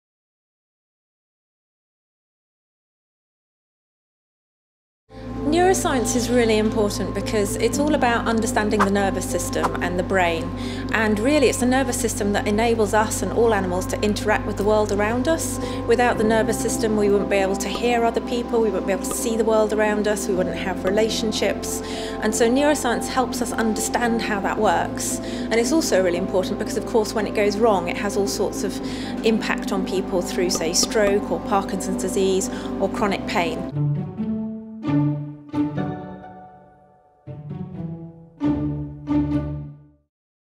Dr Anne Cooke: Why is neuroscience important?
We hear from the BNA's Chief Executive, Anne Cooke, on why neuroscience is important: “because really it is the nervous system that enables us and all animals to interact with the world around us”.